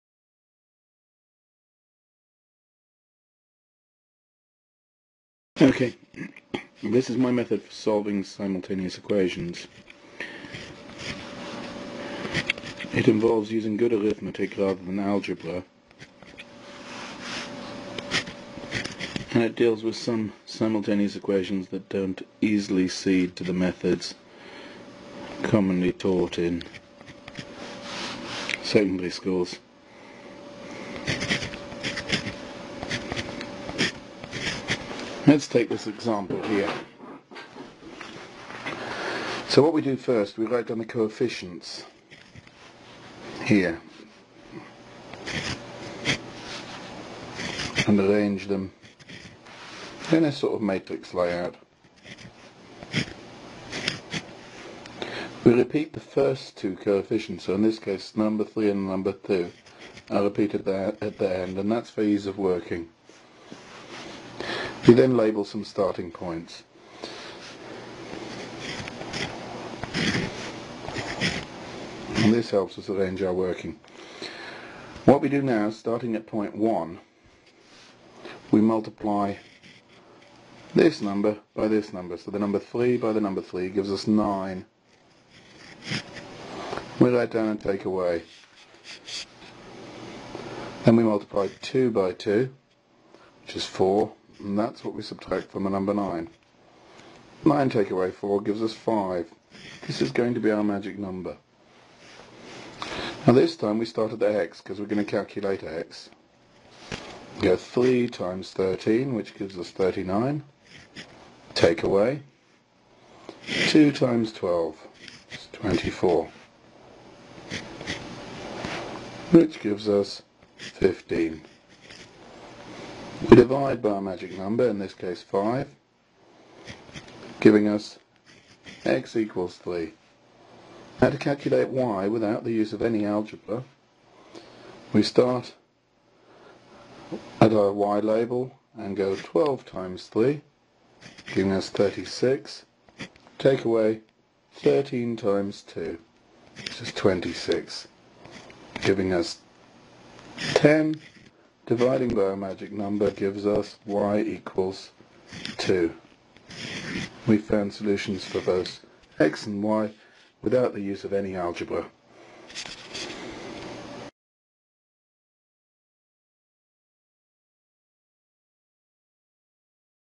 Simultaneous Equations without Algebra
Solving Simultaneous Equations without the use of algebra.
The method is aimed at GCSE students.
Advantages of the method are
1. gives solutions for both variables, without substitution or rearranging.
2. The sequence of operations is always the same. There is no deciding what to multiply by and no need to decide wether to subtract or add equations as with other methods.
3. No understanding of algebra is required.

Limitations are that it only applies to pairs of linear equations in the given format. This is however the most common format for linear equations encountered in GCSE maths exams set by the Major UK exam boards.